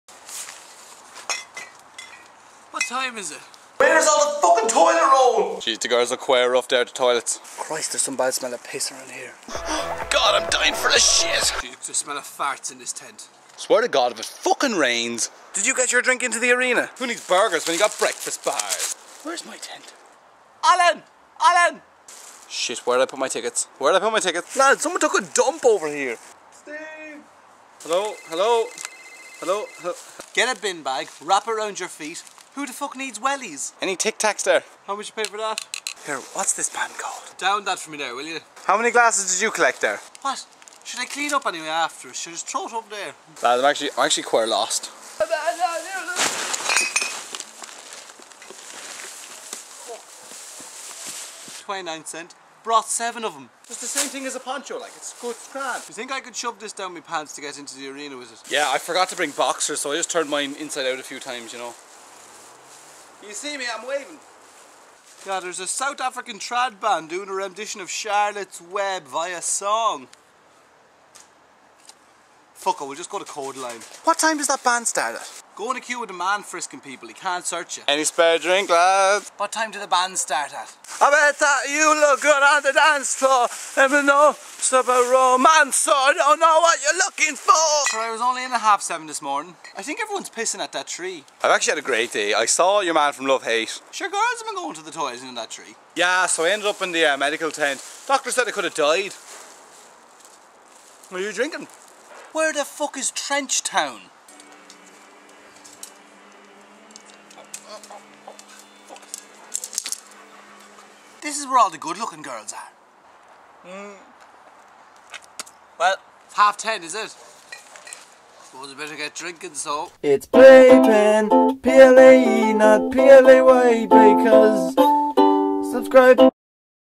Things People Say at Music Festivals
In light of the fantastic music festivals that have concluded throughout the last few months. We made this video.
NSFW language. Enjoy.